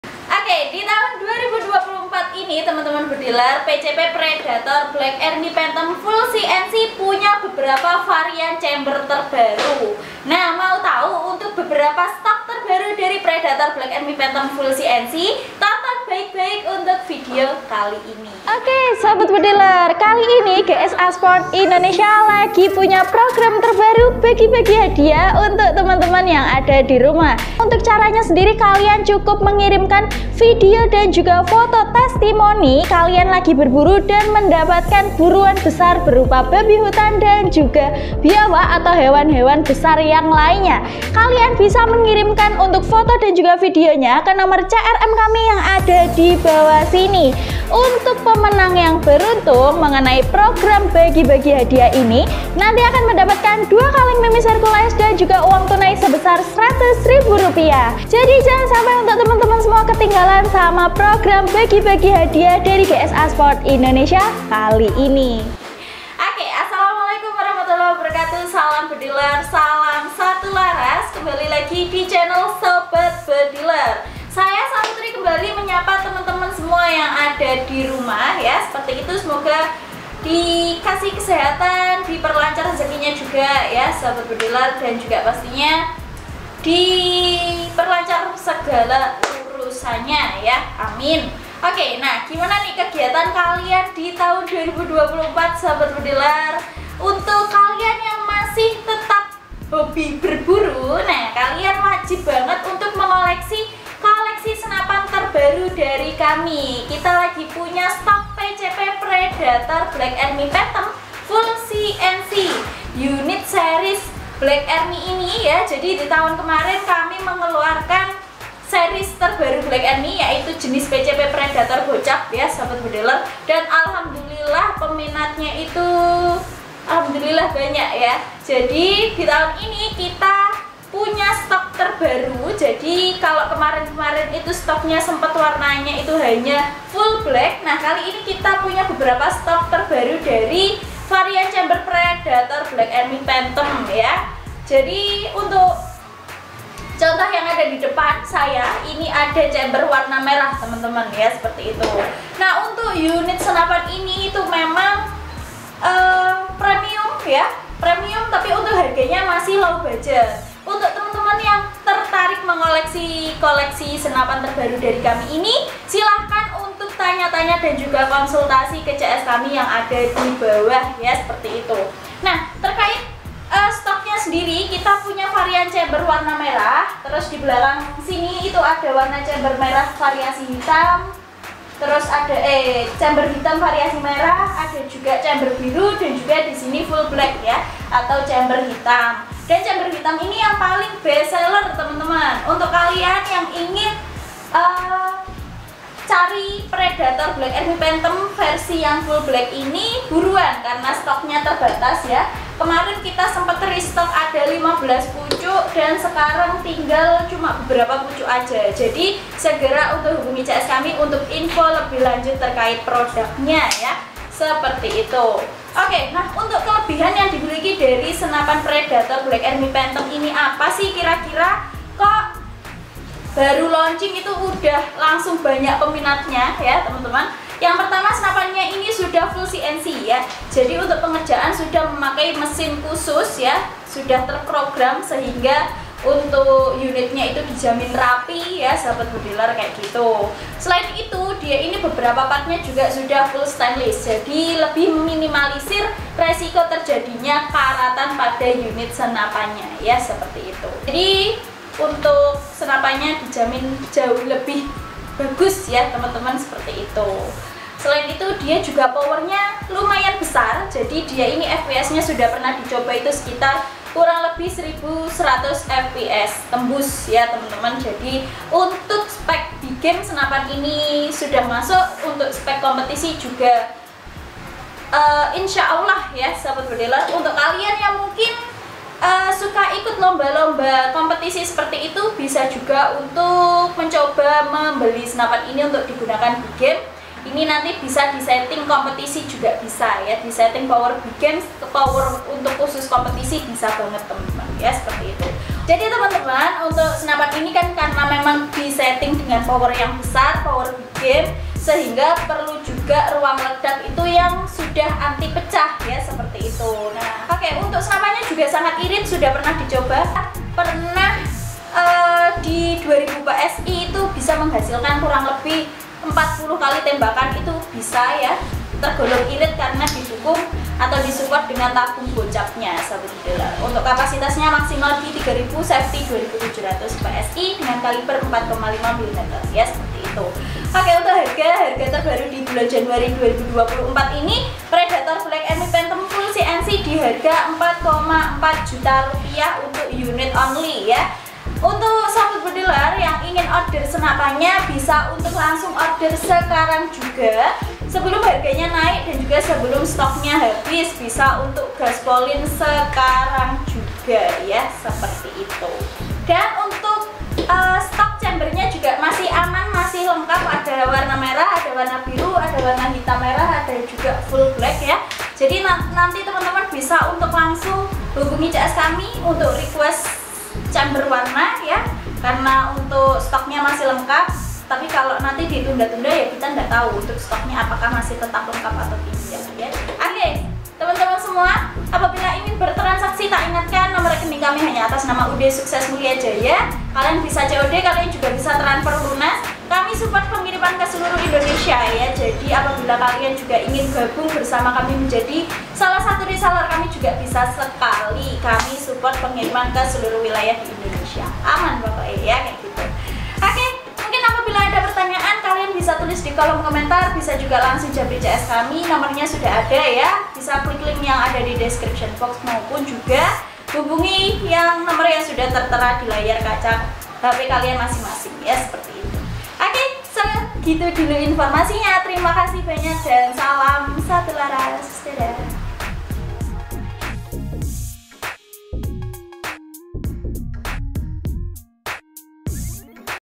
Alasan Bedilers Memilih Senapan PCP Predator Bocap Black Army Phantom Full CNC-Inilah Kelebihannya!!
Di video kali ini kami berkesempatan untuk menyampaikan

- SEMOGA BERMANFAAT -

Untuk info produk TERUPDATE & HARGA TERBAIK/konsultasi,yuk sapa kami di wa Customer Service kami, klik link berikut

Senapan angin telah menjadi pilihan populer di kalangan pecinta olahraga menembak atau hobi berburu. Salah satu model yang sangat diminati oleh para bedilers adalah Senapan PCP Predator Bocap Black Army Phantom Full CNC. Berikut adalah beberapa alasan mengapa senapan ini menjadi pilihan utama bagi banyak bedilers dan kelebihan yang dimilikinya.